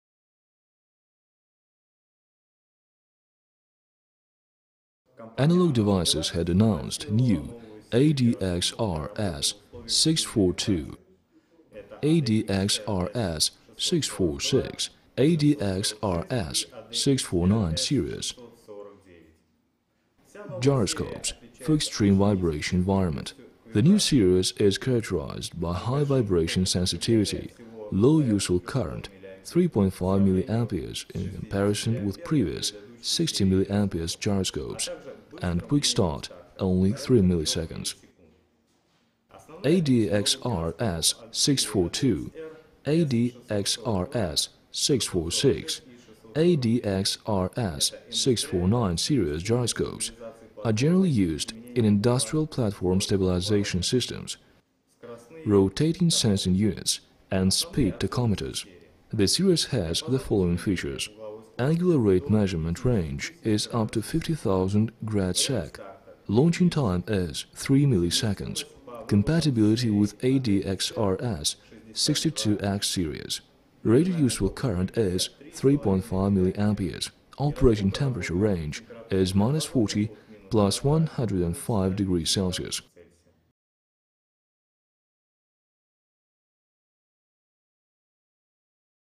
ADXRS64x Gyroscopes for Extreme Vibration ...
ADXRS64x Gyroscopes for Extreme Vibration Environment       Analog Devices had announced new ADXRS642, ADXRS646, ADXRS649 series gyroscopes for extreme vibration environment.       The new series is characterized by high vibration sensitivity, low useful current (3.5mA in comparison with previous 60mA gyroscopes) and quick start (3 ms).       ADXRS642, ADXRS646, ADXRS649 series gyroscopes are generally used in industrial platform stabilization systems, rotation sensing units and speed tachometers.       The series has the following features:       •angular rate measurement range is up to 50000 grad/sec (ADXRS649);       •launching time is 3 ms;       •compatibility with ADXRS62x series;       •rated useful current is 3.5mА;       •operating temperature range is  -40...+105°С